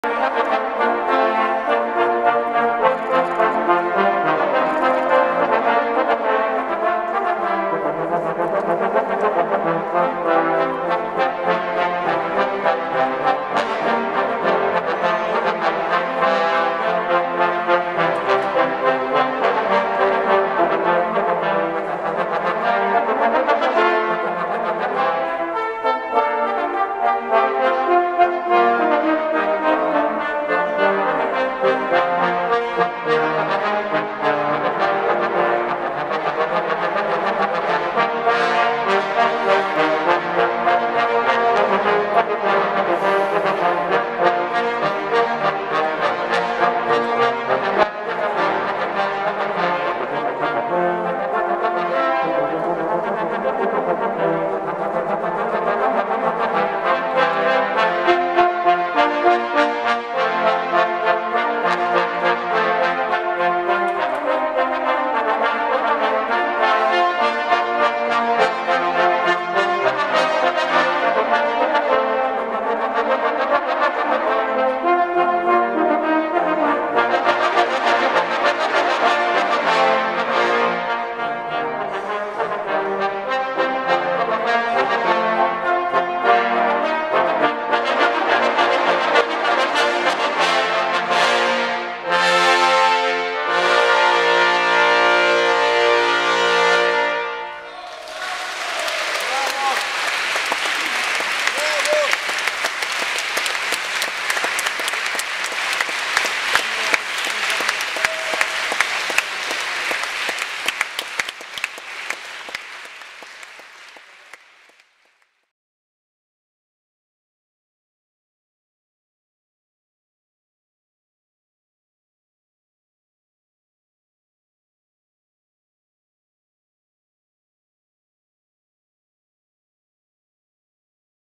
Telemann trombones choir
2-ой Всероссийский конкурс ансамблей духовых инструментов г.Казань